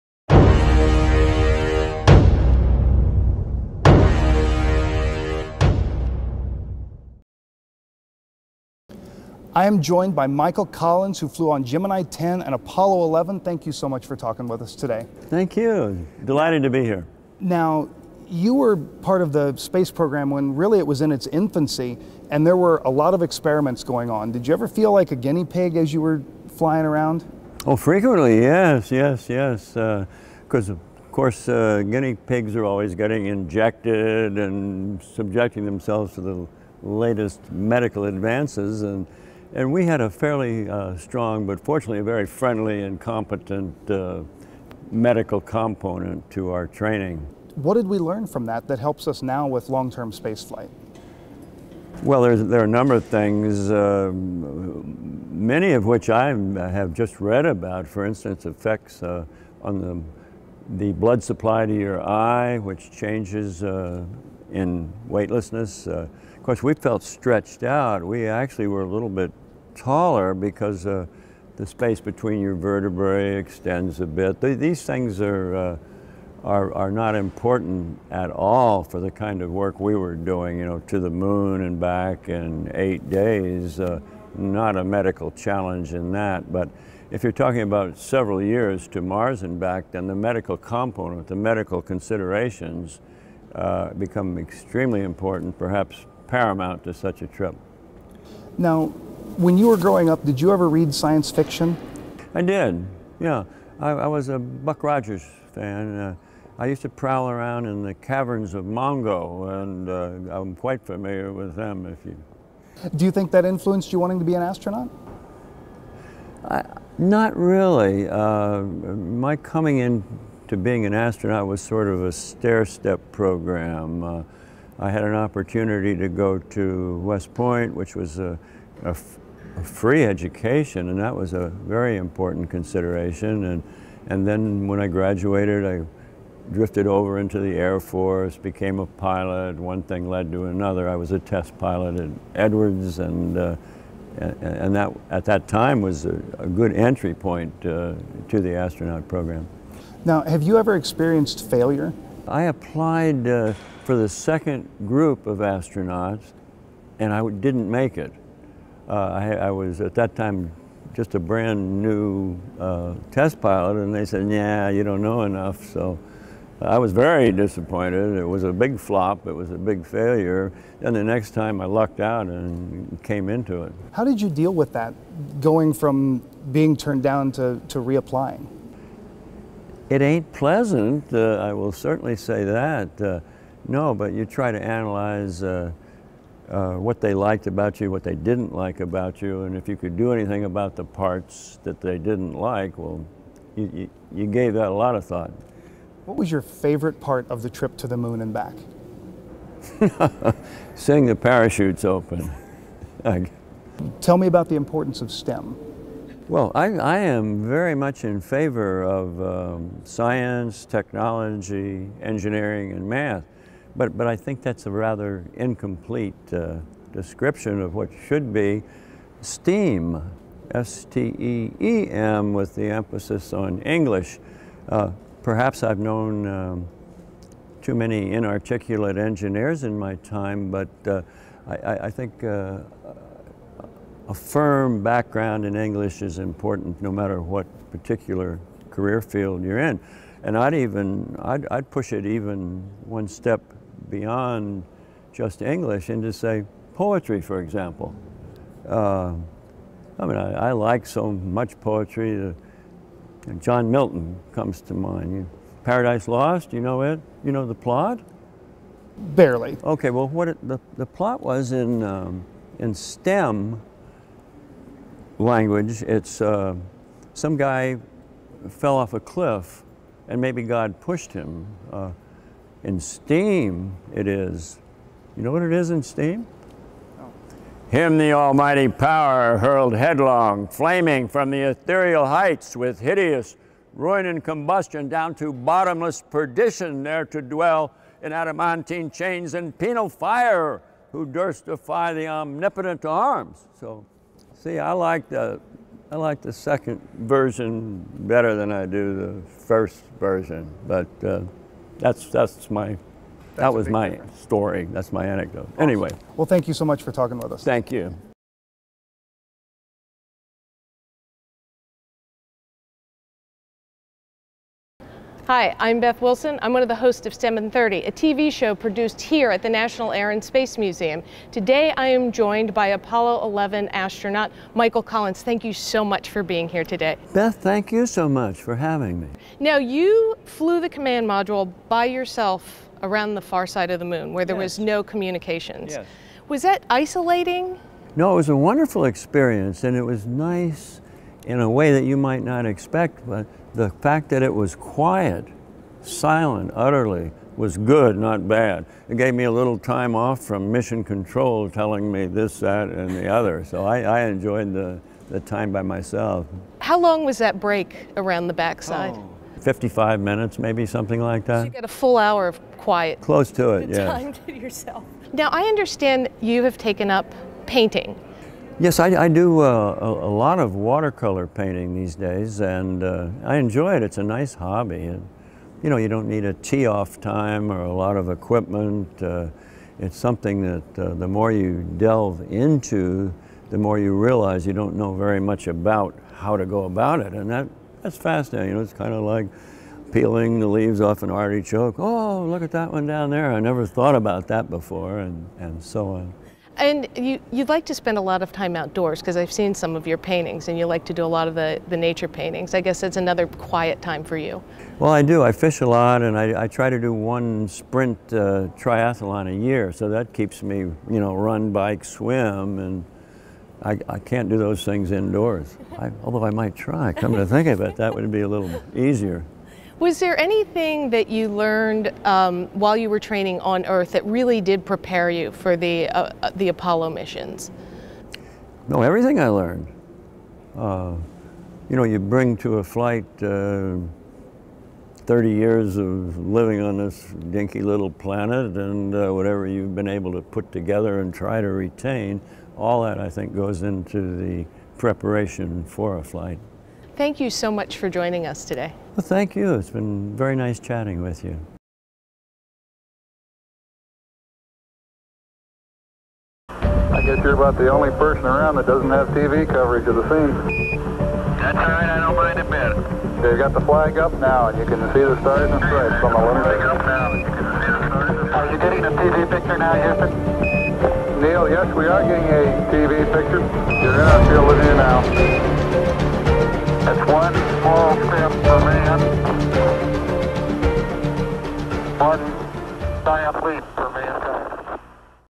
An Interview With Apollo 11 Astronaut: Michael Collins
Michael Collins, is an American former astronaut and test pilot. Selected as part of the third group of fourteen astronauts in 1963, he flew into space twice. His first spaceflight was on Gemini 10, in which he and Command Pilot John Young performed two rendezvous with different spacecraft and Collins undertook two EVAs.

Credit: NASA